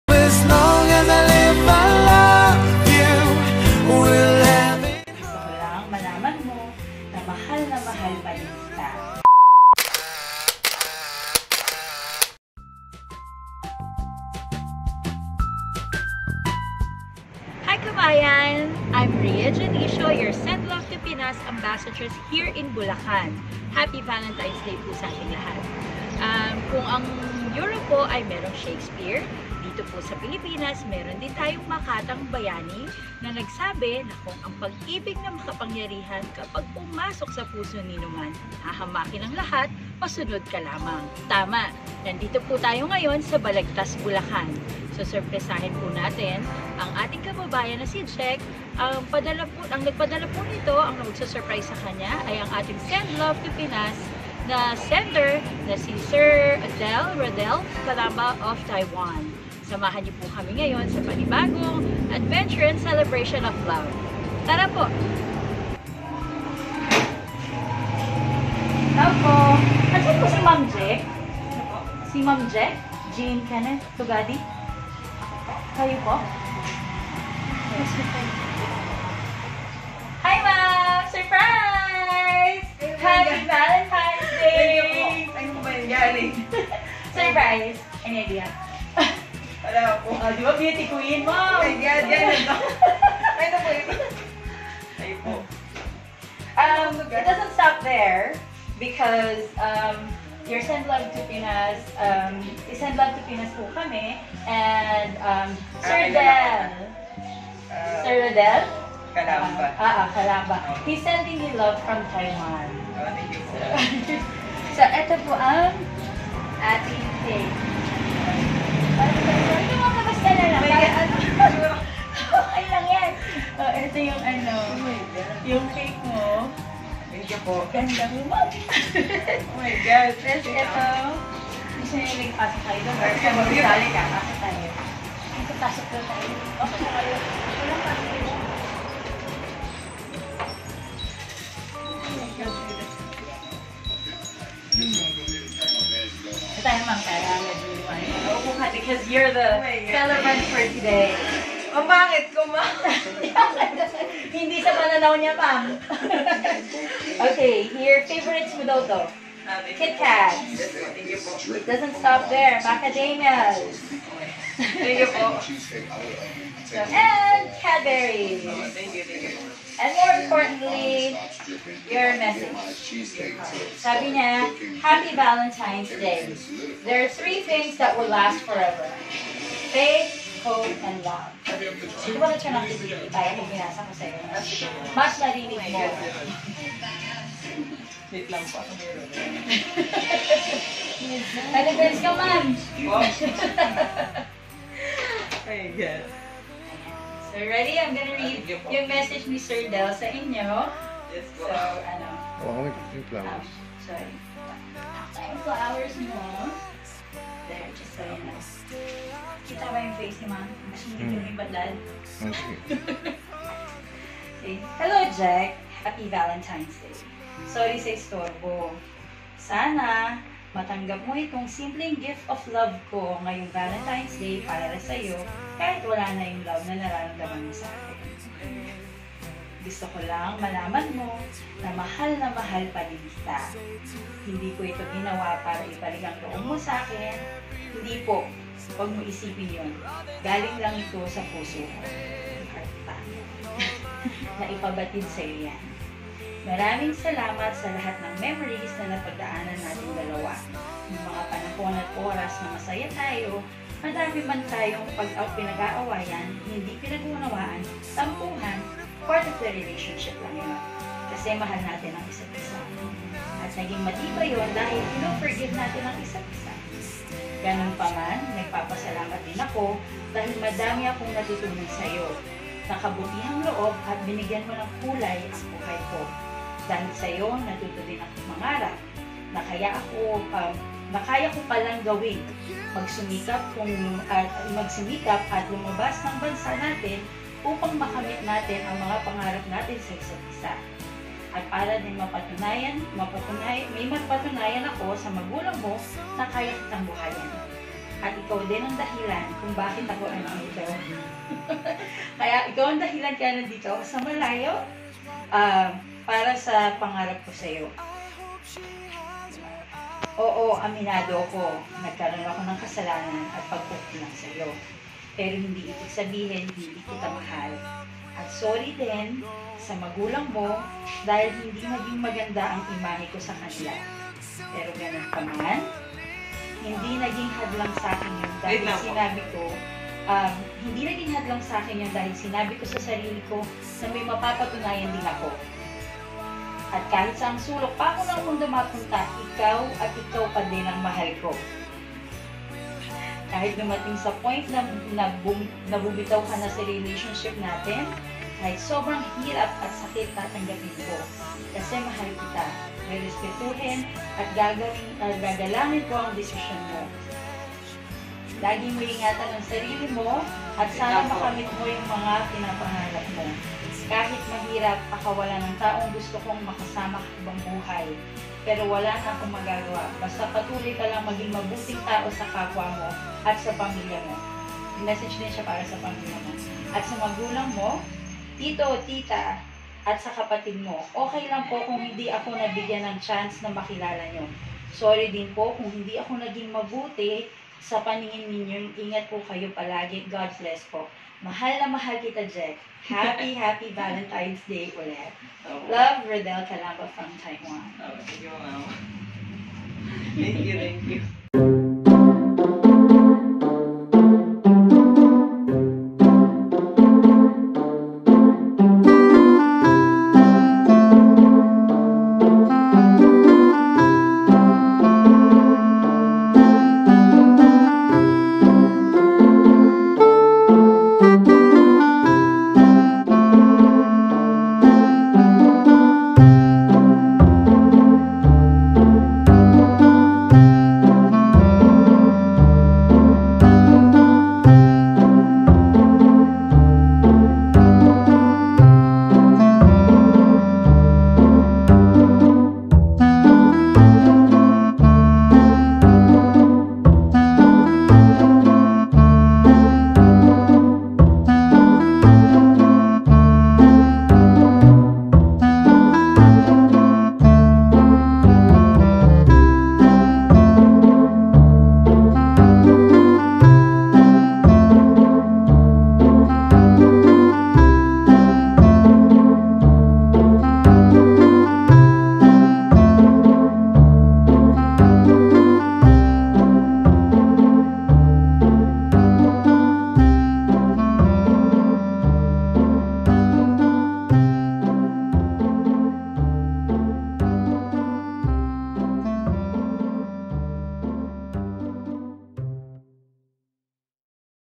DAHIL SAYO NAGING MAKULAY ANG MUNDO KO 🤗😍
Nasorpresa si ma'am Jean sa mga inihandang sorpresa sa kanya ng pinakamamahal niya na si sir Rodel💕💕

🎥Let's watch our Send Love Ambassador Ria delivered gifts and surprises sealed with love 💕

Sending Love from Taiwan 🇹🇼 to Pinas 🇵🇭

To: JEAN KENNETH TUGADI
From: RODEL CALAMBA

Sendlove2pinas delivered BasicPackage as follows

🍫1k worth of cMacademia/Kitkat/Cad

*Surprise Delivery
*Photos & video of loved ones precious
reaction

SPECIAL MESSAGE:

hello jek 😊 happy valentines sorry sa istorbo , sana matanggap mo itong
simpleng gift of love ko ngayung valentine's para sayo kahit wala na yung love
 na nararamdaman mo sakin gusto ko lang malaman mo na mahal na mahal
pa din kita hindi ko ito ginawa para ibalik ang loob mo sakin hindi po wag
mong iisipin yun galing lang po ito sa puso ko ❤ na ipabatid sayo . Maraming
 salamat sa lahat ng memories na napag daanan nateng dalawa nung mga
panahon at oras na masaya tayo madami man tayung pag aaway hindi pag
uunawaan, tampuhan part of relationship lang yun kasi mahal naten isat isa
at naging matibay yun dahil pinofirgive naten ang isat isa . Ganon pa man nag
 papasalamat pa din ako dahil madami akong natutunan sayo na kabutihang
loob at binigyan mo ng kulay yung buhay ko dahil sayo natuto din ako
mangarap na kaya ko palang gawin sinikap kong lumabas ng bansa naten
upang makamit naten ang mga pangarap naten sa isat isa at para din may
mapatunayan ako sa magulang mo na kaya kitang buhayin at ikaw din ang
dahilan kung bakit ako andito sa malayo sayo para sa pangarap ko sayo.
Oo Aminado ko nagkaroon ako ng kasalanan at pag kukulang sayo pero hindi
ibig sabihin hindi kita mahal at sorry din sa magulang mo dahil hindi naging
maganda ang imahe ko sa kanila pero ganun pa man hindi naging hadlang
sakin yun dahil sinabi ko sa sarili ko na may mapapatunayan din ako .
At kahit san sulok pako ng mundo mapunta ikaw at ikaw pa din ang mahal ko.
Kahit dumating na sa point na bumitaw kana sa relationship naten kahit
sobrang hirap at sakit tatanggapin ko kasi mahal kita rerespetuhin ko at
gagalangin ko ang desisyon mo .. Lagi mo iingatan sarili mo at sana makamit

HOW?!

Thanks!
Spread Love! 💕